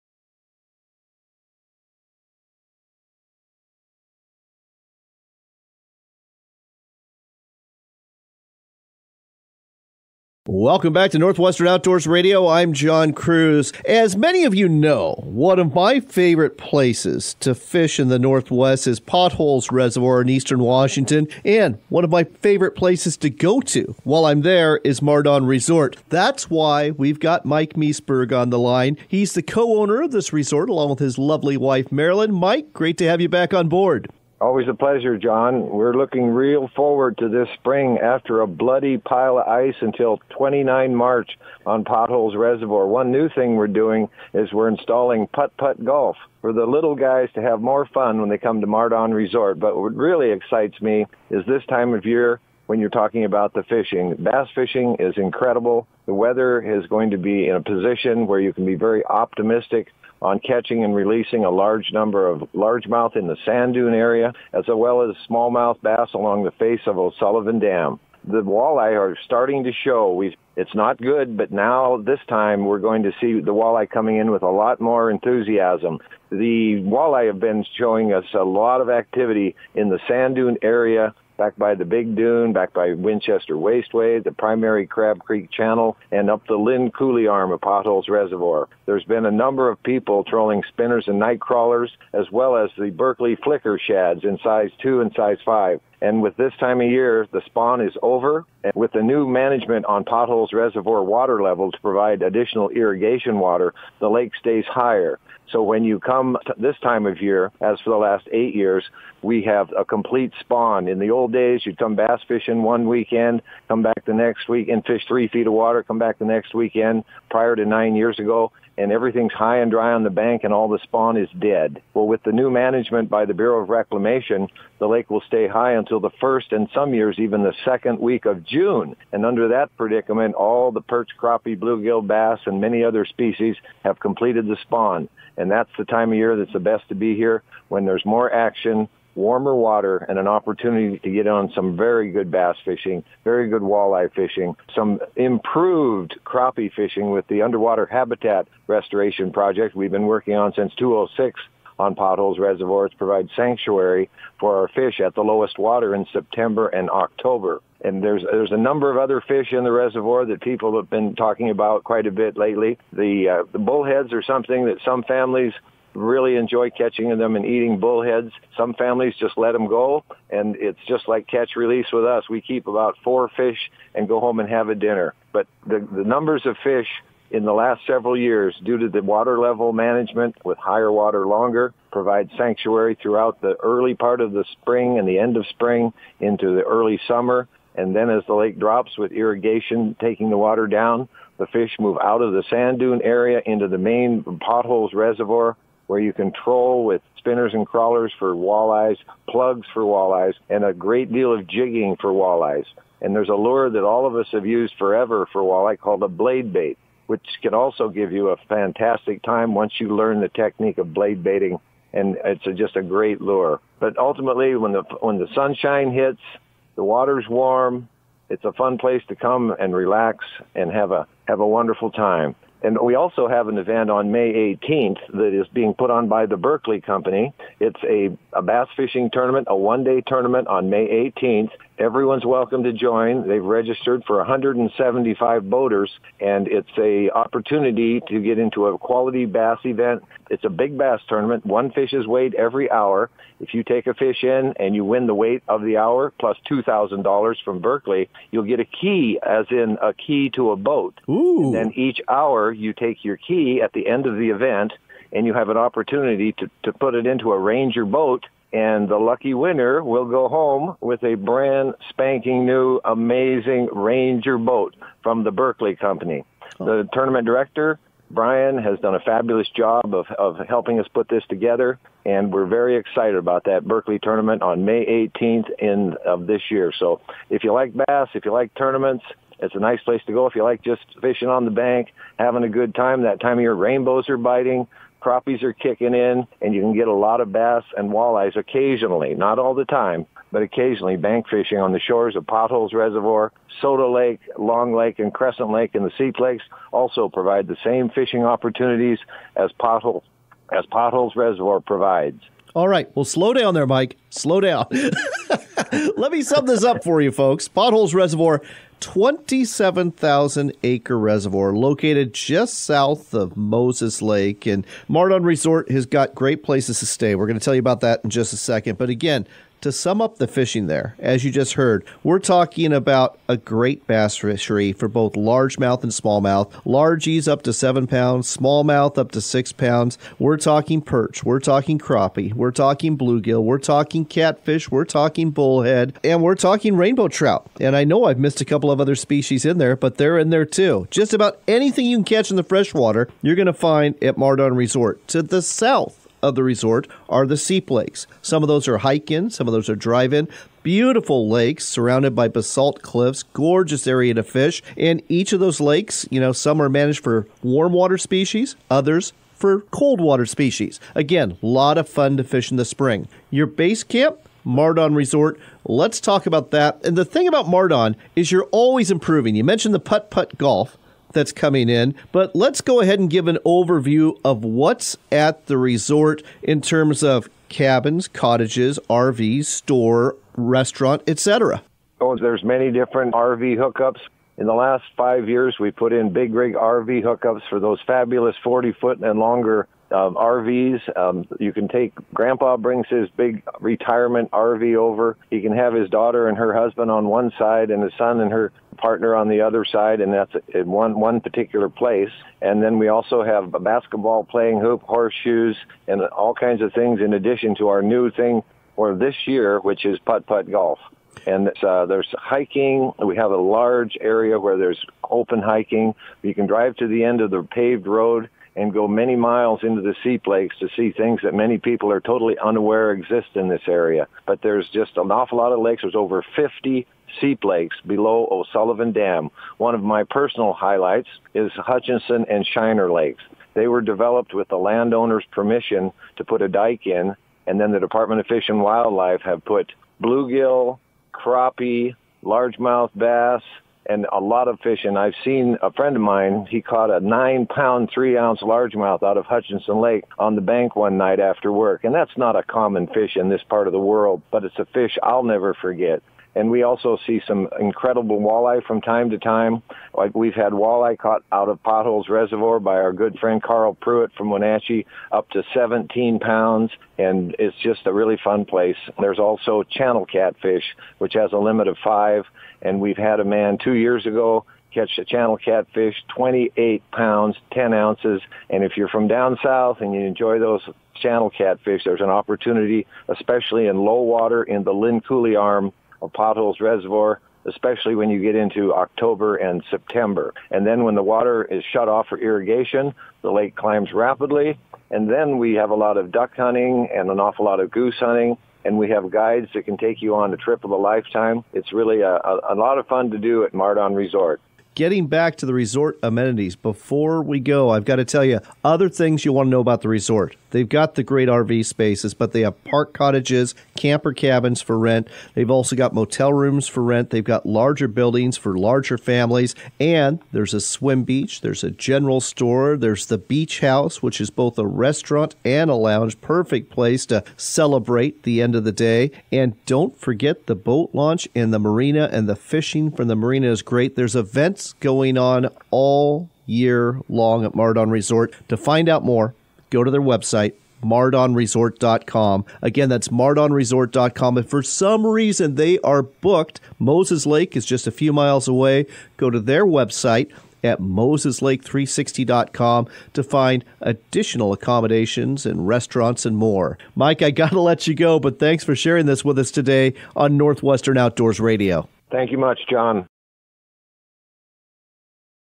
Fishing and Fun at Mar Don Resort
Mike Meseberg, the co-owner of Mar Don Resort in Eastern Washington, talks about the extensive improvements made here in recent years and the great fishing available at Potholes Reservoir along with the nearby Seep Lakes.  Air Date - May 11, 2019